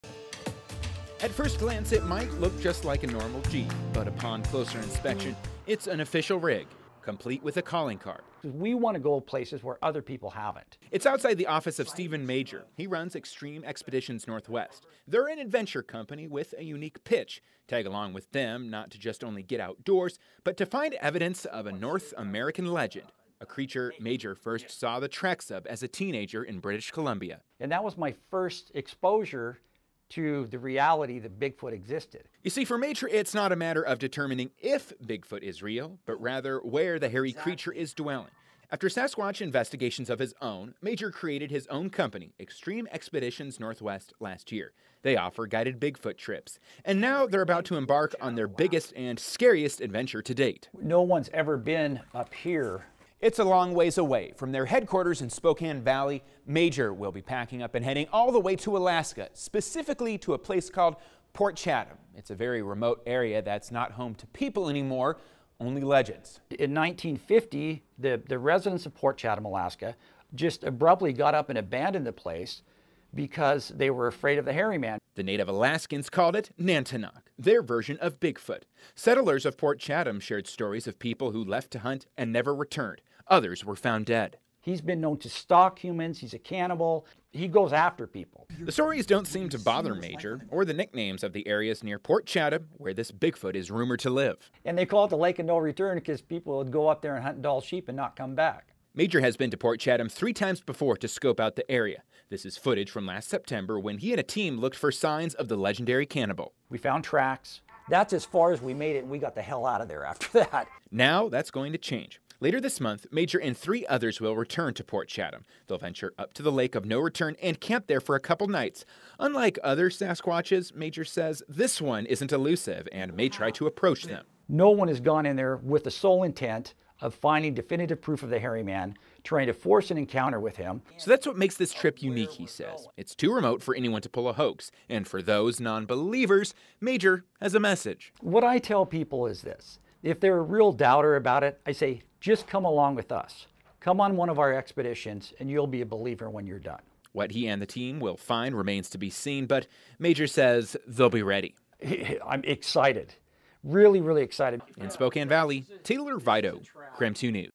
'Come on one of our expeditions and you'll be a believer': Spokane Valley group searches for Bigfoot
KREM Reporter Taylor Viydo caught up with a group from Spokane Valley that works to track Bigfoot.